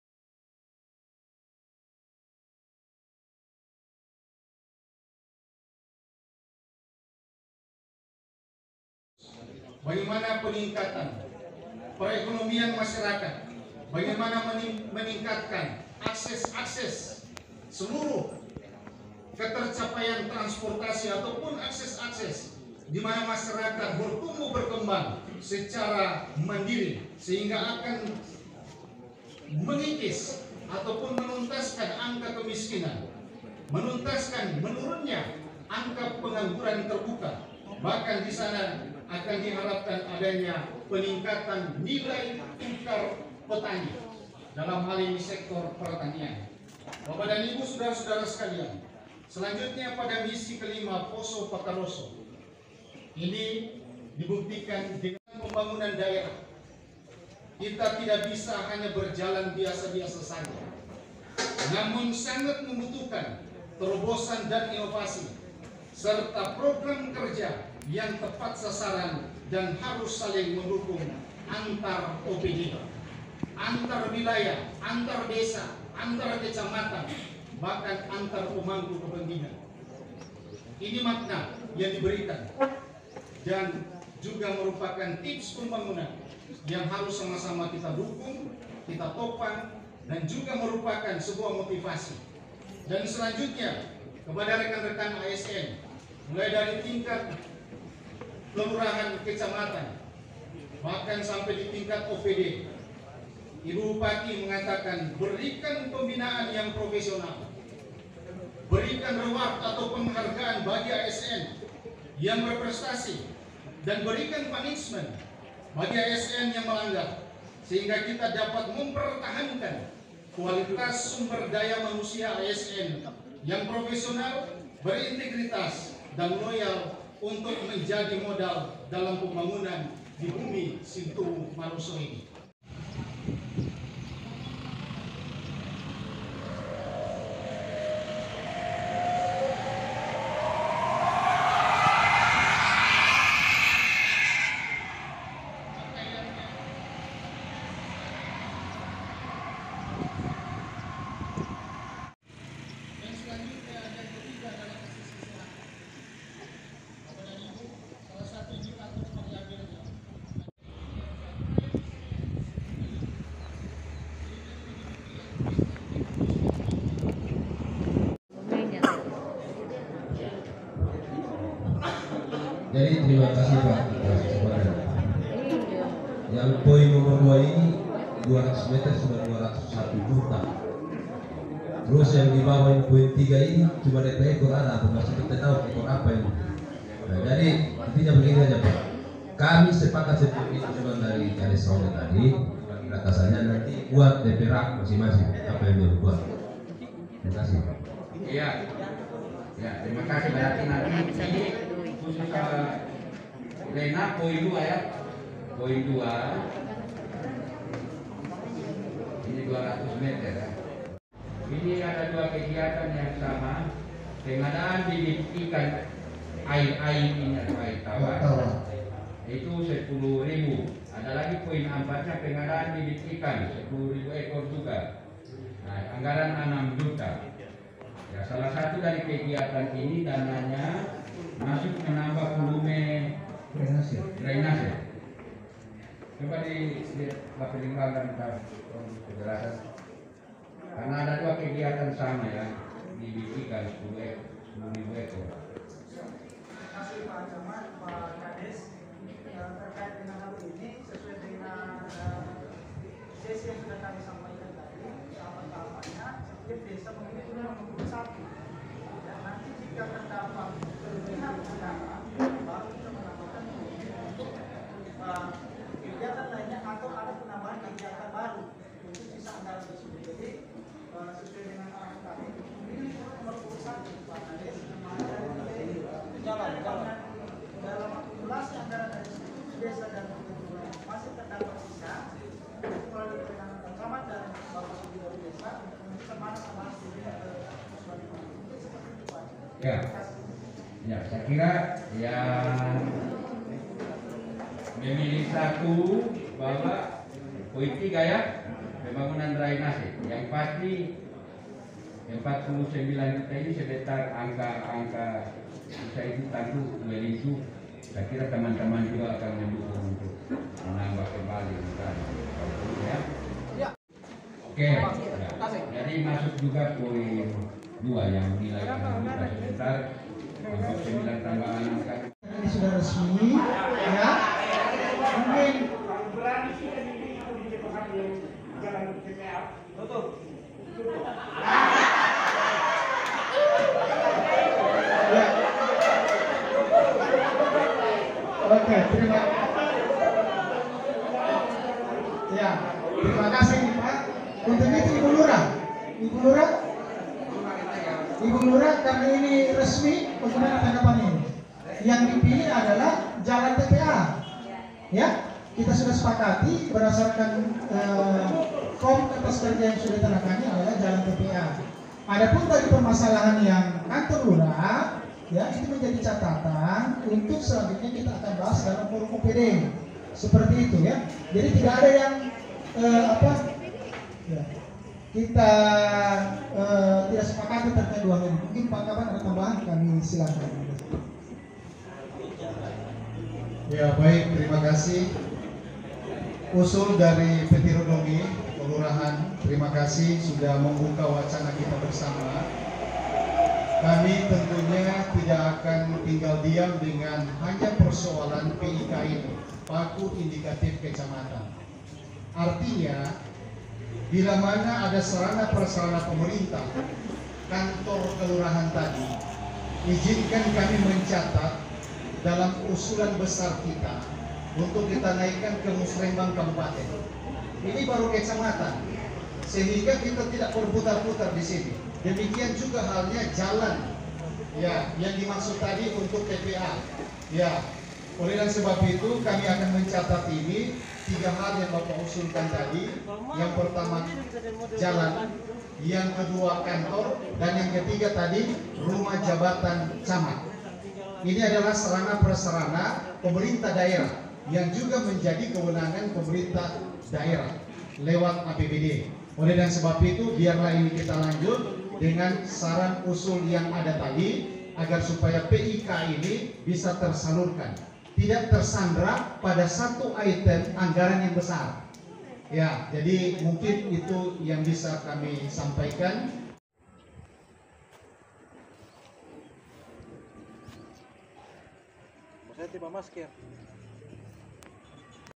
Musrembang Kec. Pamona Utara#Pemda Kab.Poso
Musrenbang adalah salah satu pijakan pemerintah dalam melakukan pembangunan dimana semua usulan diterima serta disaring sesuai skala prioritas yang dilakukan oleh para OPD. seperti yang dilaksanakan oleh Pemerintah Daerah Kabupaten Poso benar-benar dilaksanakan sesuai dengan kebutuhan yang sangat Prioritas dari desa dan Kelurahan se-Kecamatan Pamona Utara, sehingga bisa mendukung dan selaras dengan kebijakan pemerintah Kabupaten Poso .. mari dukung chanel ini dengan like, komen dan subscribe Sulewana Chanel agar bisa berkembang.. terimakasih..